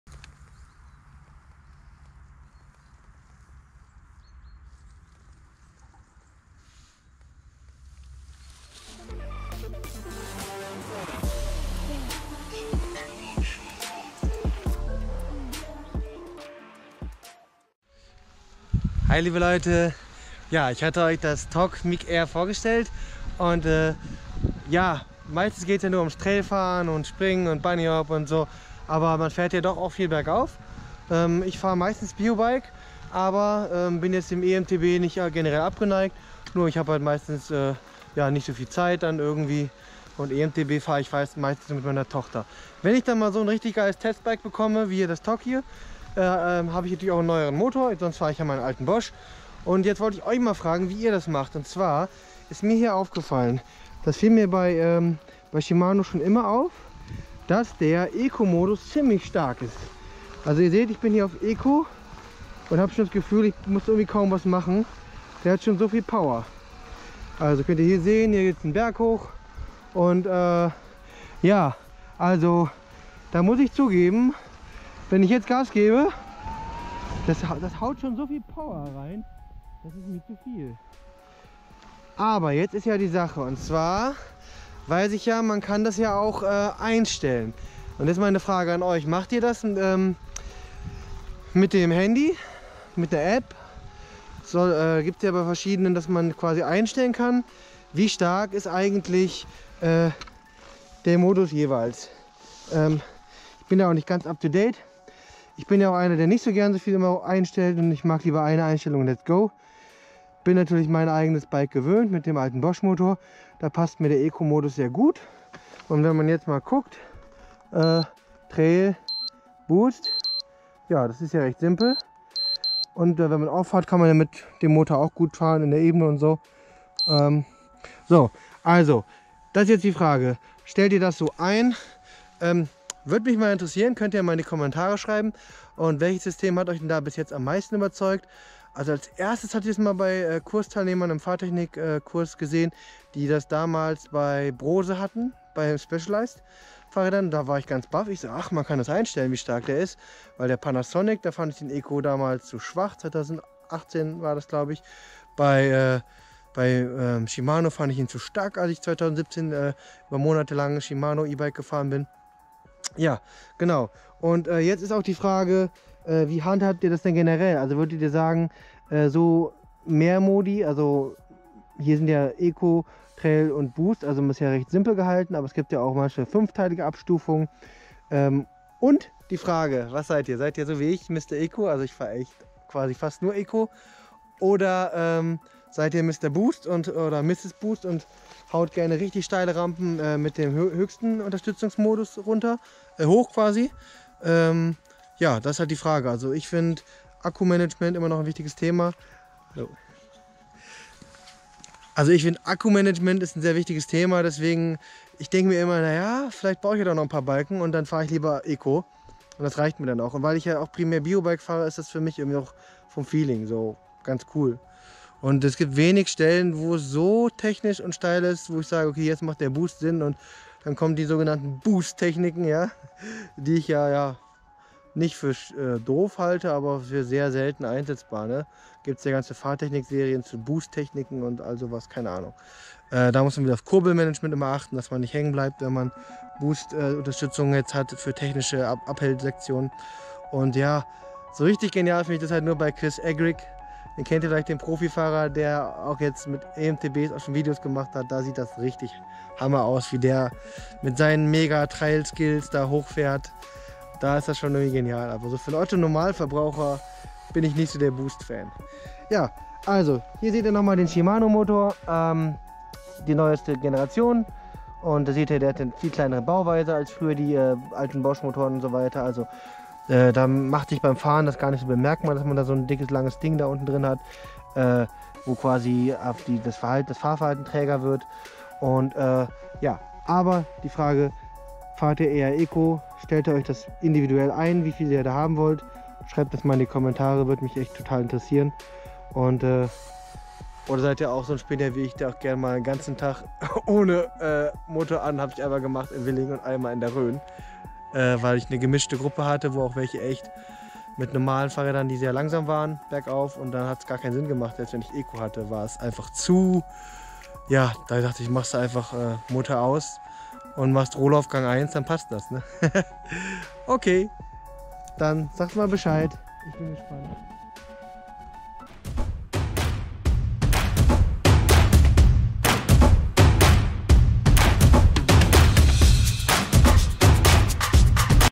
ECO, TURBO oder was am besten? E-MTB Talk mit Marc Brodesser
In welchem Modus fahrt Ihr am liebsten? Und habt Ihr die Level individuelle eingestellt per App? :) INFO: Hier kannst Du Marc als Coach buchen ODER lass Dich von Marc zum Fahrtechnik-Coach ausbilden, hier alles zur BEP Ausbildung